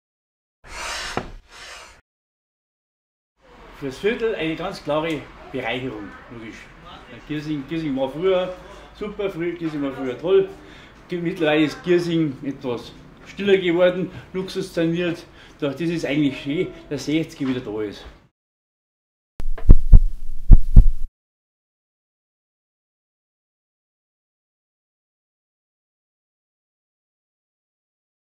Trepperlwirt unterstützt Sechzig im Sechzger
Zahlreiche Kneipen, Restaurants und Läden in und um Giesing unterstützen Sechzig im Sechzger. Hier im Video zu sehen: Der Trepperlwirt in der Tegernseer Landstraße 119 in Obergiesing.

Alle weiteren Unterstützer findet ihr unter: www.

Werde auch du Unterstützer/Unterstützerin und Teil von Sechzig im Sechzger. ein und setze Dich mit Deiner Stimme für einen dauerhaften Verbleib des TSV 1860 München in Giesing ein.